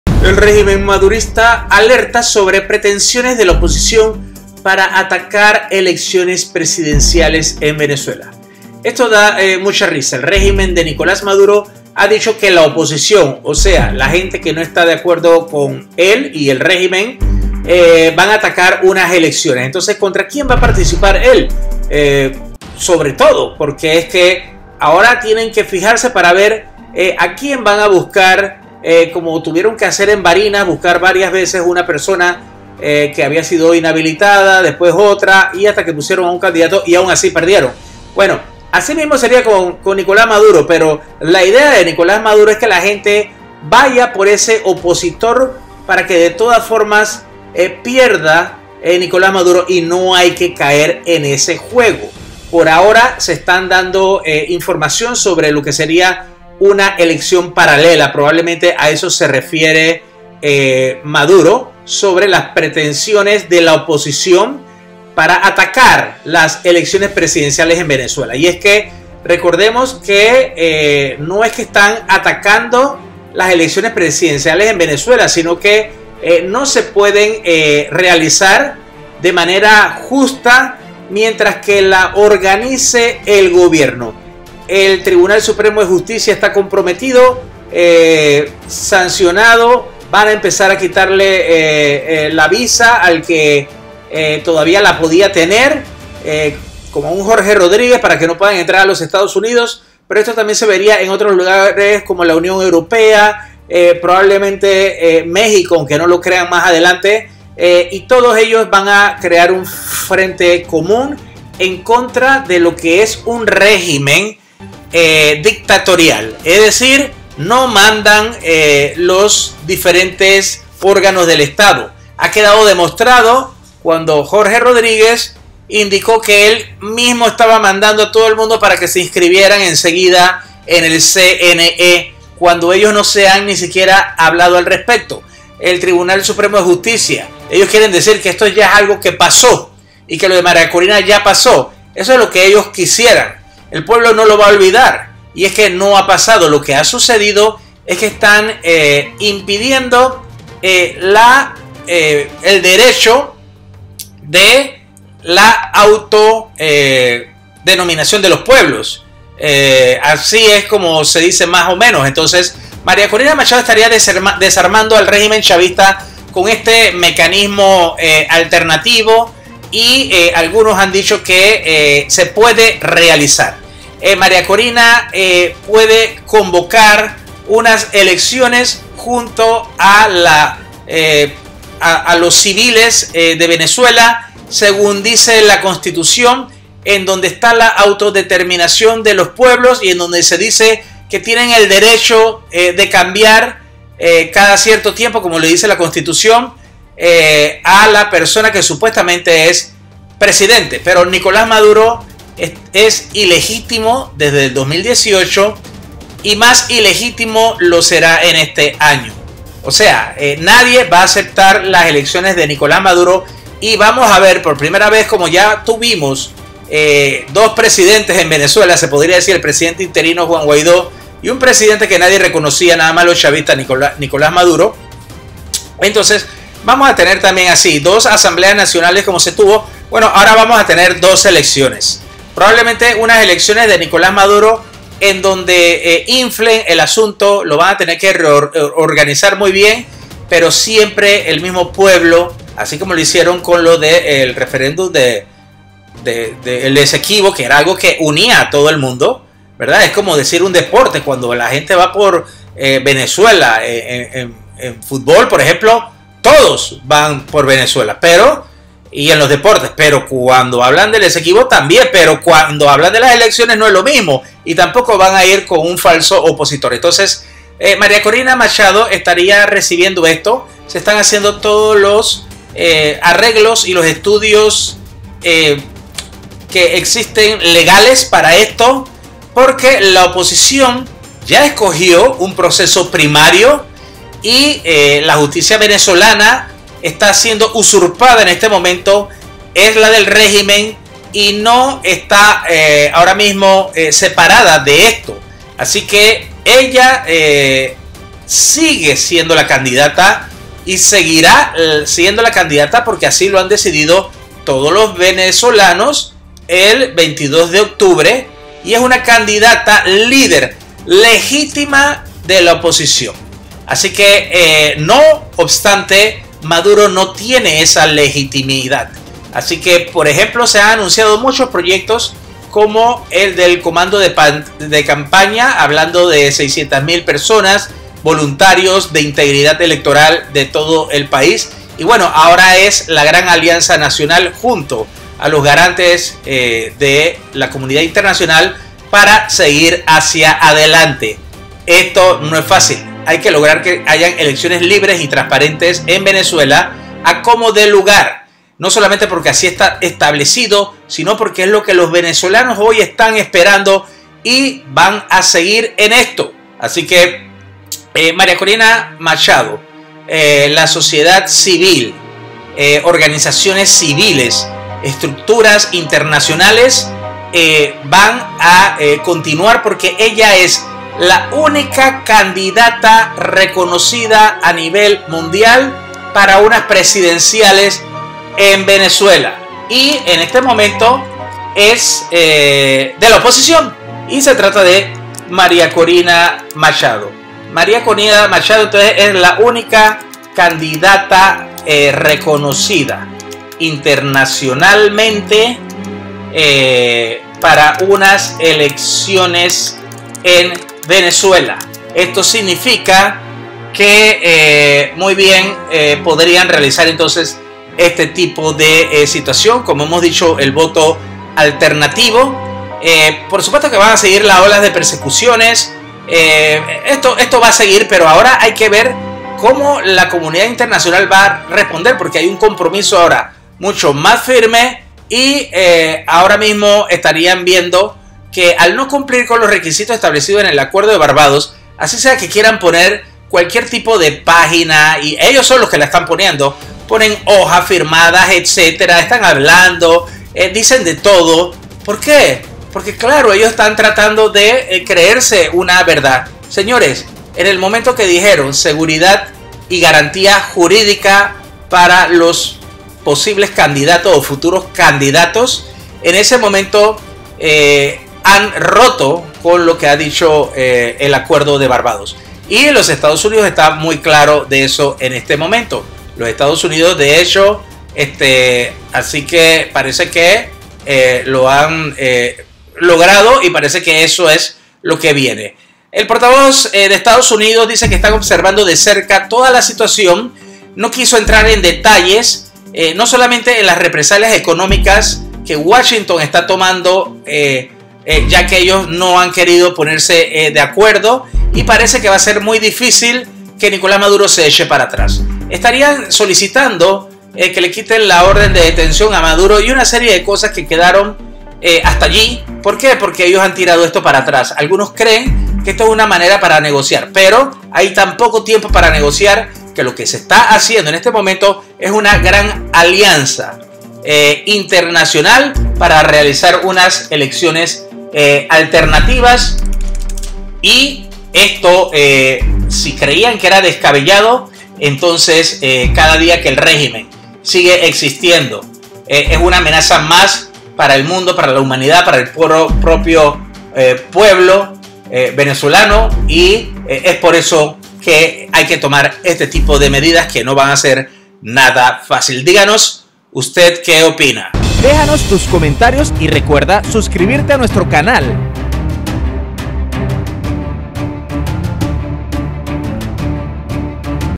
Maria Corina candidata a PRESIDENTE de VENEZUELA reconocida por el MUNDO Maduro es ilegitimo
La unica candidata a PRESIDENTE de VENEZUELA reconocida por el MUNDO es Maria Corina Eleccion ALTERNA. Articulo 5 permite el derecho de la poblacion a cambiar al presidente.
Video de Militares Otan Rusia Colombia CPI Fanb by VOA
Maduro y Maria Corina Machado, Guaido fotos y videos by Prensa Presidencial y by Asamblea Nacional public domain y VOA